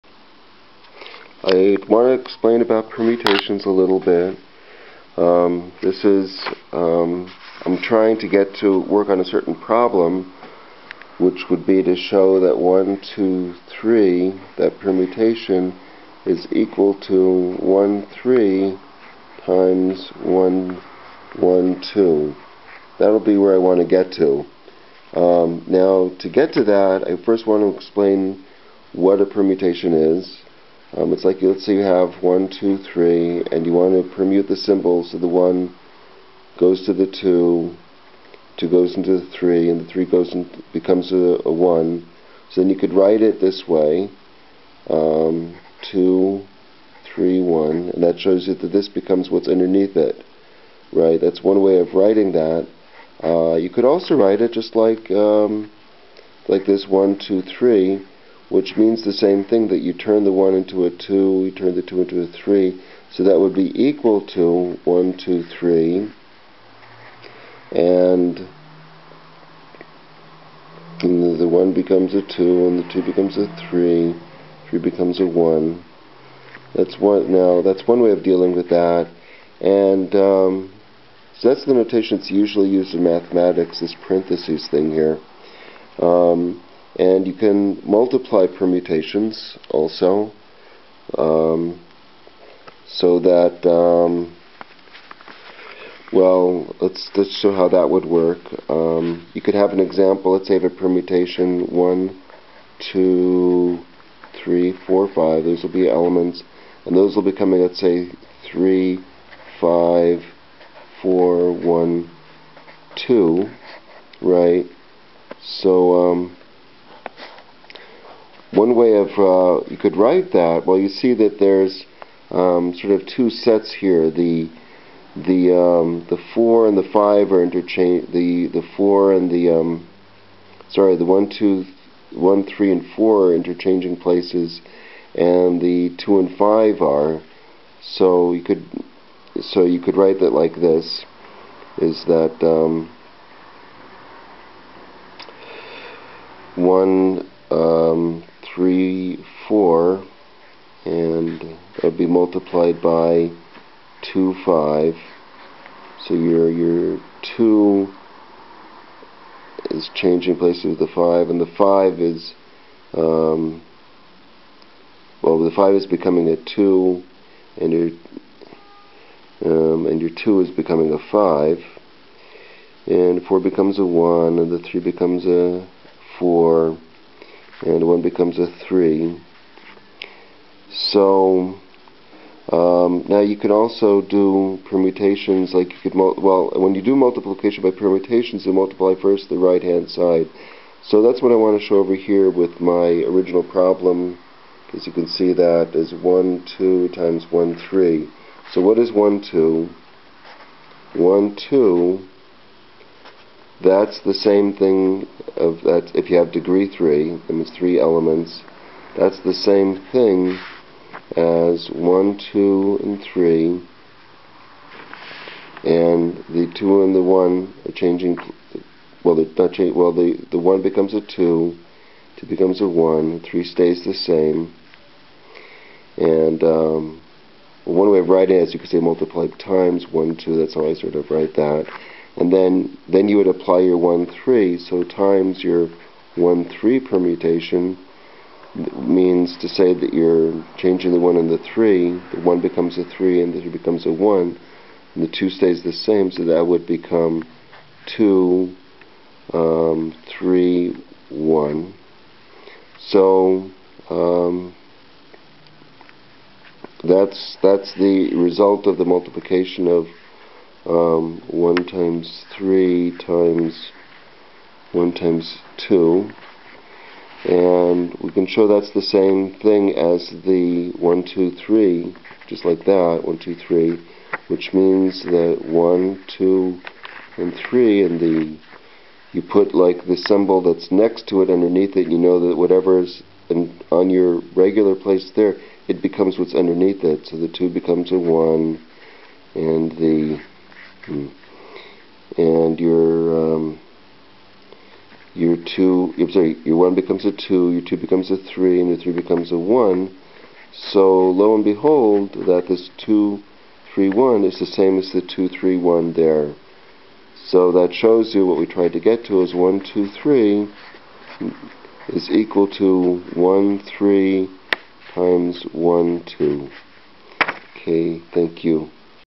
permutations
permutations is a subject that comes up in linear algebra and group theory so i thought i should make a small video about it